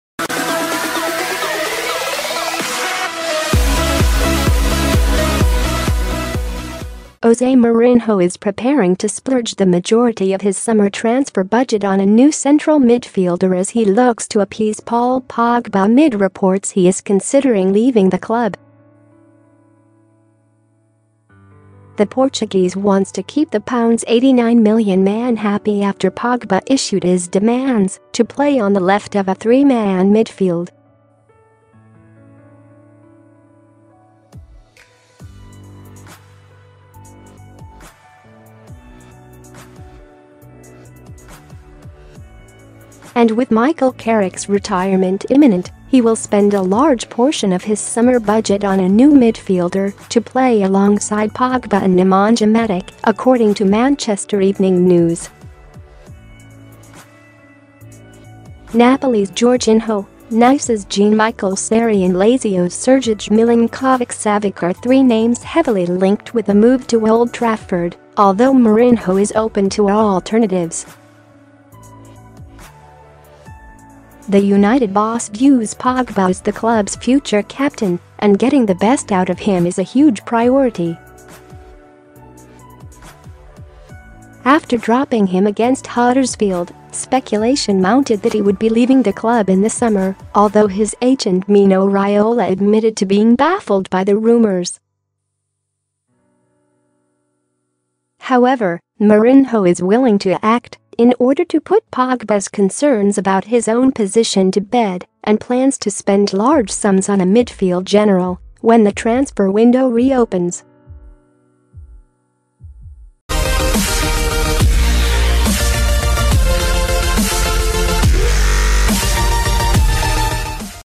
Man Utd transfer news: Jose Mourinho to appease Paul Pogba by using summer budget on midfield signin
Man Utd transfer news: Jose Mourinho to appease Paul Pogba by using summer budget on midfield signing
Jose Mourinho is preparing to splurge the majority of his summer transfer budget on a new central midfielder as he looks to appease Paul Pogba amid reports he is considering leaving the club. The Portuguese wants to keep the £89million man happy after Pogba issued his demands to play on the left of